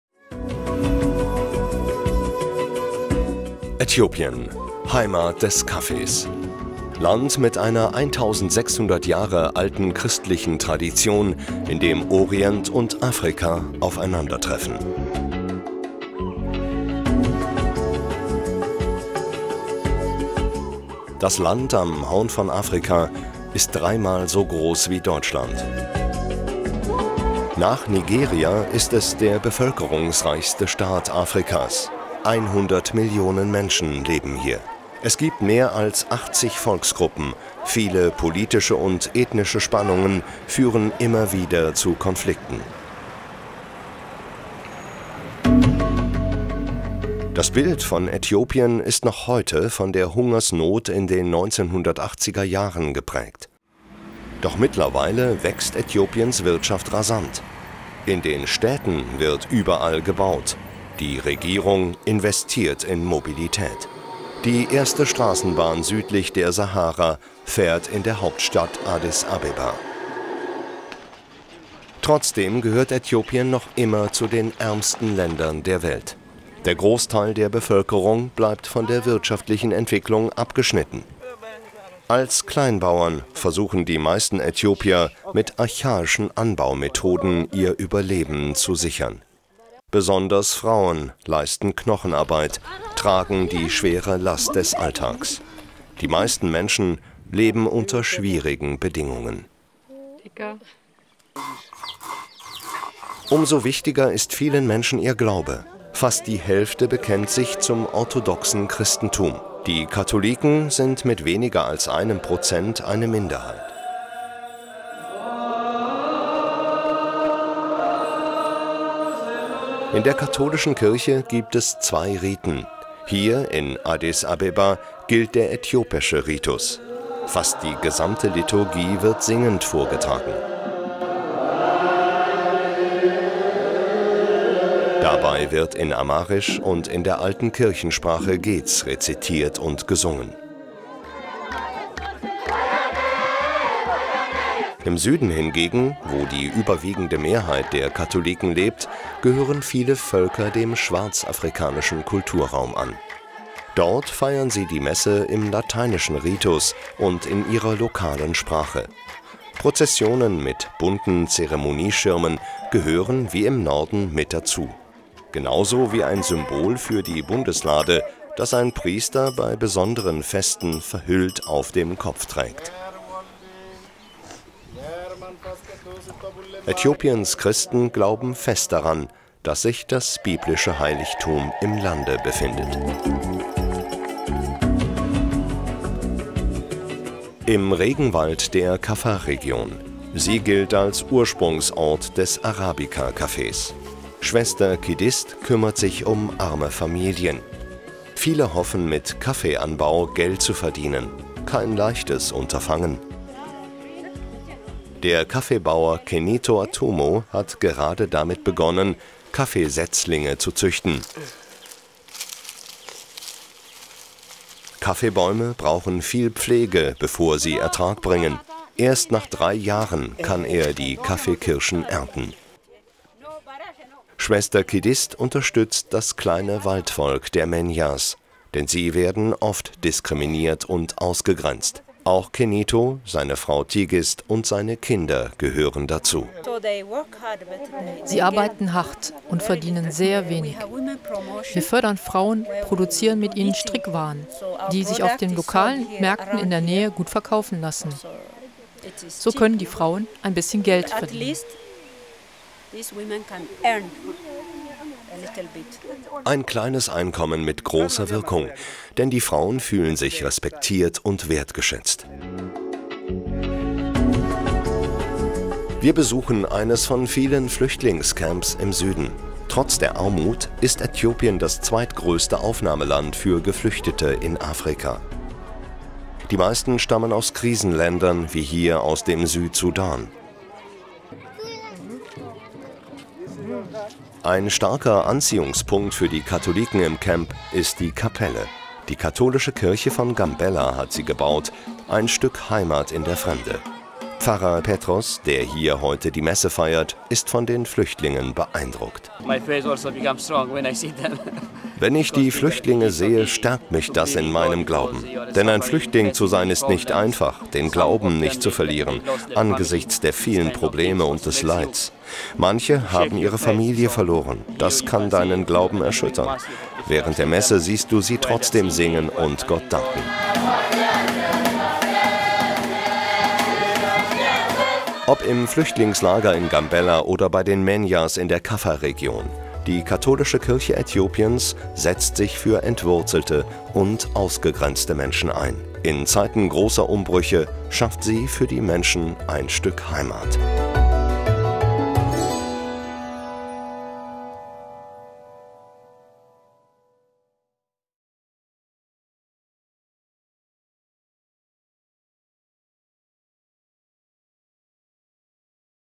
Weltmissionssonntag 2018: Aktionsfilm zu Äthiopien │missio
Beim Weltmissionssonntag 2018 ist Äthiopien das Partnerland der missio-Aktion. Der Aktionsfilm führt in das Land zwischen Afrika und Orient ein, zeigt wie sich die katholische Kirche Äthiopiens für ausgegrenzte und entwurzelte Menschen einsetzt. Im Süden Äthiopiens, in der Region Kaffa, in der der Arabica Kaffee angebaut wird, setzt sich die Ordensschwester Kidist für das kleine Volk der Menjas ein, das von anderen Volksgruppen ausgegrenzt wird. Im Südwesten des Landes, an der Grenze zum Südsudan hilft Fr. Tesfaye Petros südsudanesischen Flüchtlingen, ihren Glauben zu leben und die Hoffnung nicht zu verlieren.